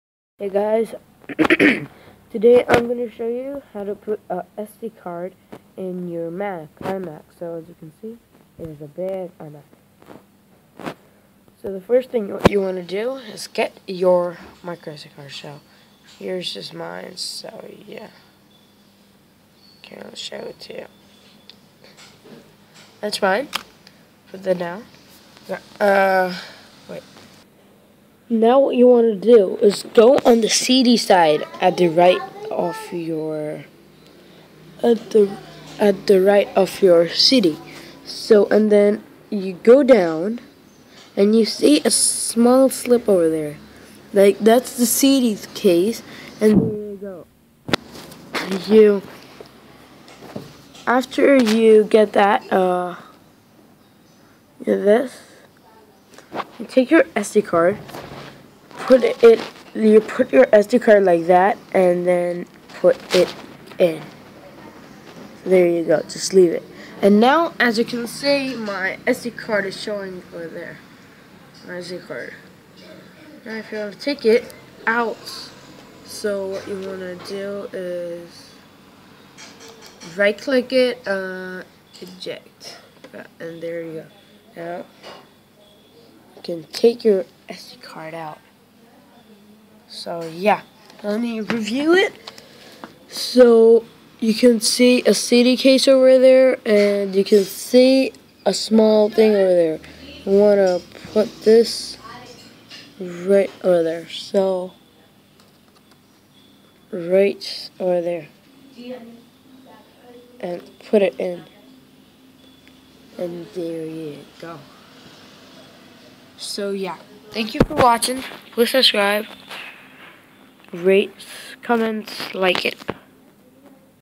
how to put a sd card on a imac
hey guys people were asking me to make a video about this so i made it.